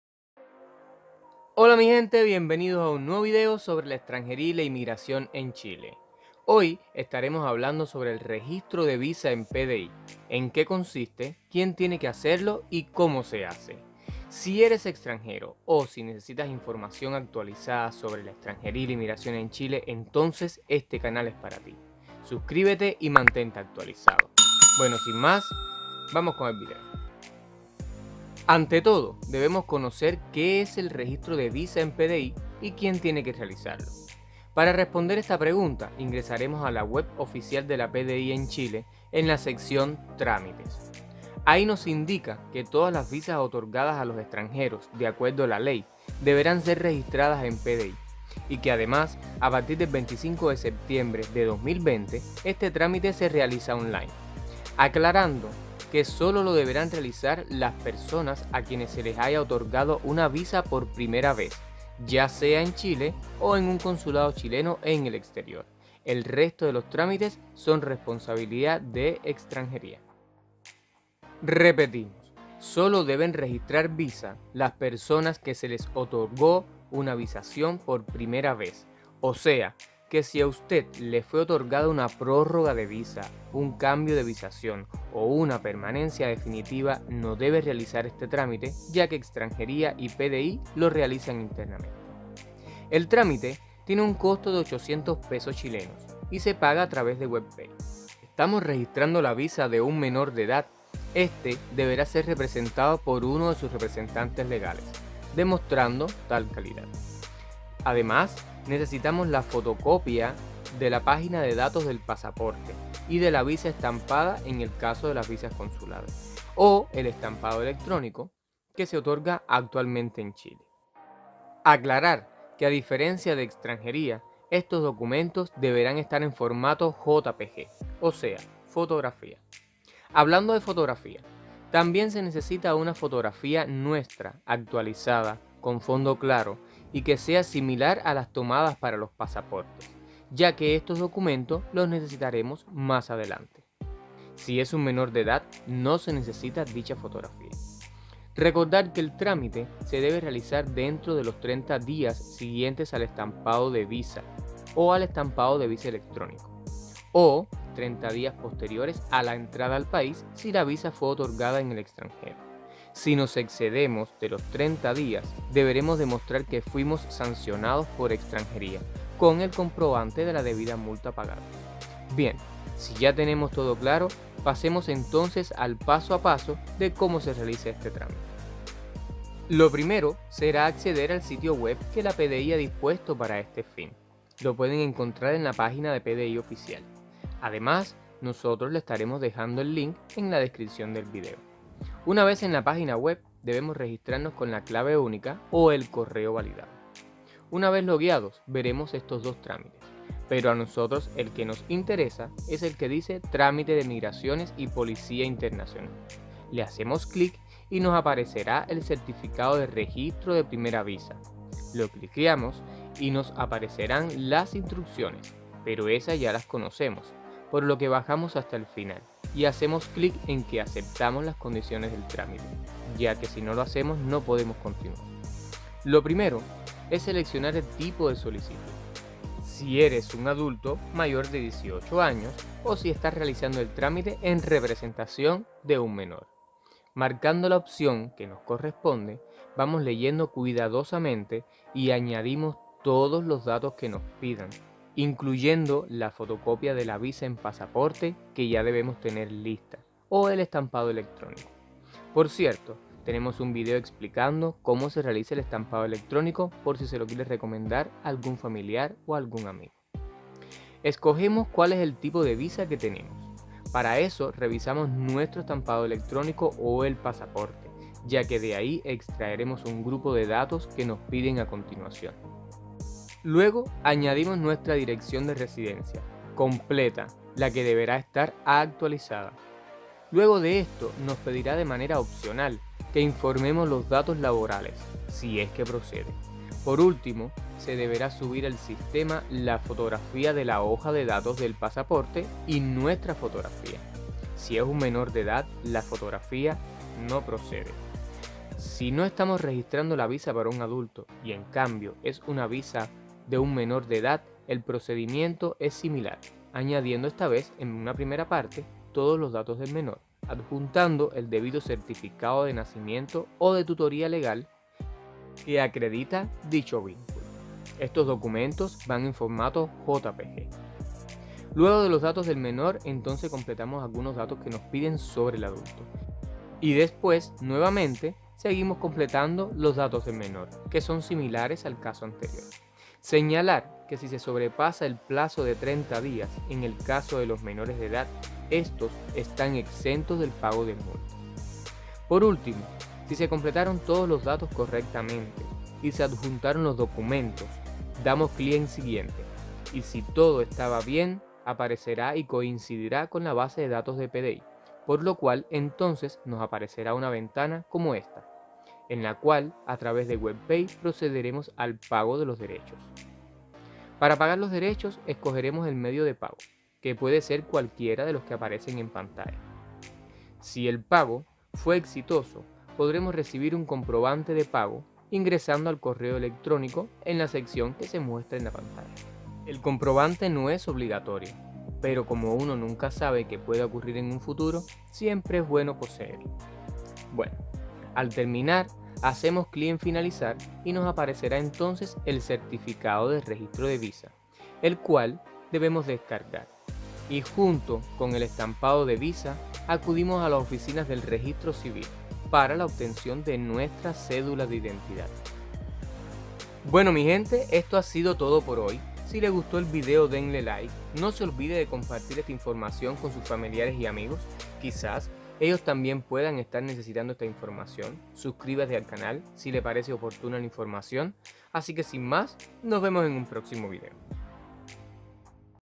Cómo registrar una visa en PDI Virtual. Fácil y Rápido.
Cómo registrar una visa en PDI Virtual. En que consiste este trámite y quien tiene que hacerlo.

Link para ingresar a PDI virtual y poder hacer el registro de visa:
Sobre los logos utilizados.
Que se encuentra bajo la siguiente  licencia (CC) ¨Atribución¨ de acuerdo a los lineamientos comunicacionales oficiales para plataformas digitales: